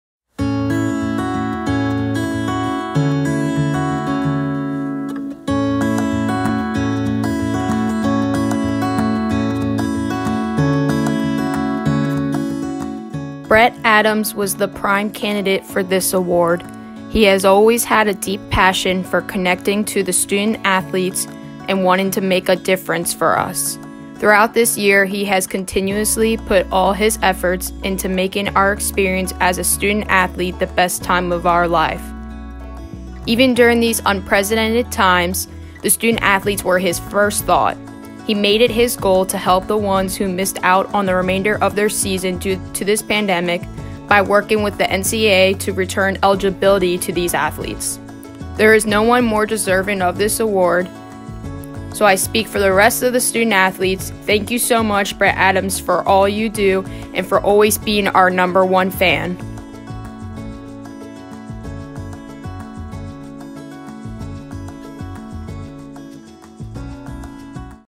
2019-20 SAAC Staff Appreciation Award
The Stevenson Student-Athlete Advisory Committee has selected athletic director Brett Adams as the winner of this year's Staff Appreciation Award.